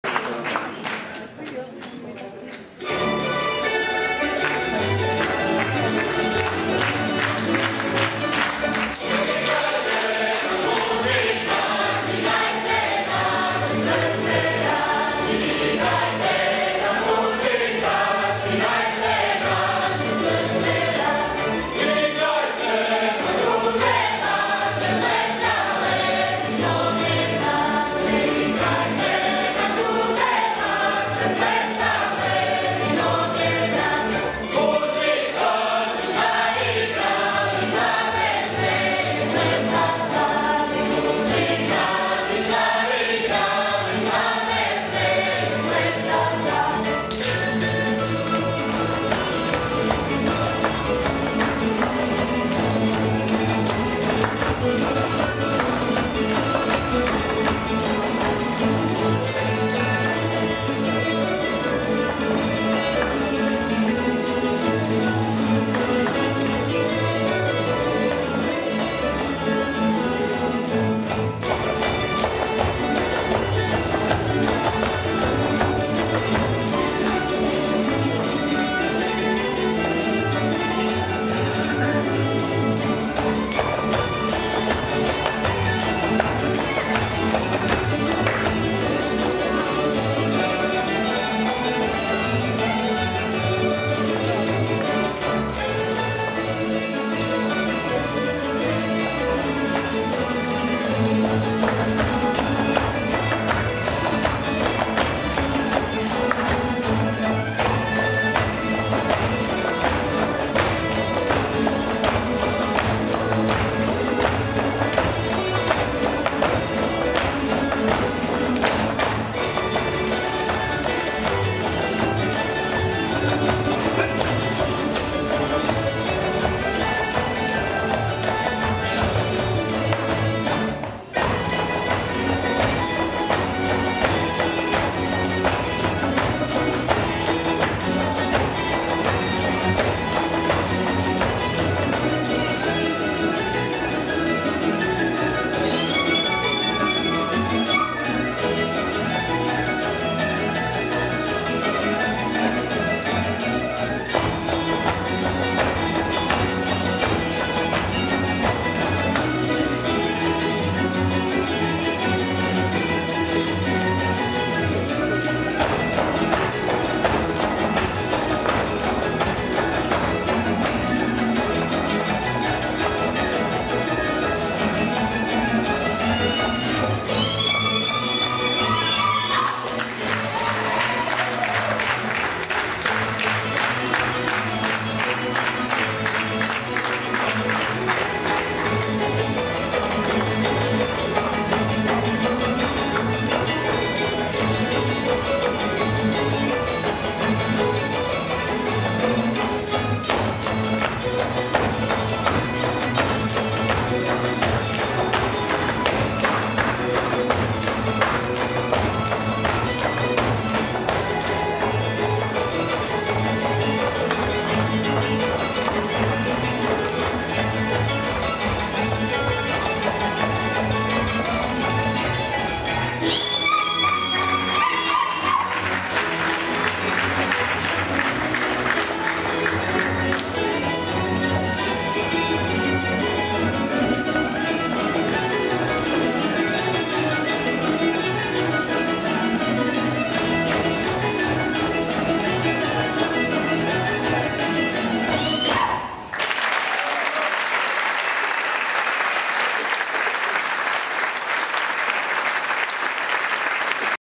FA"TUROPOLJE",Velika Gorica : Pjesme i plesovi Zagorja
NOVOGODIŠNJA  FOLKLORNA  VEČER
Zagreb,january 2011.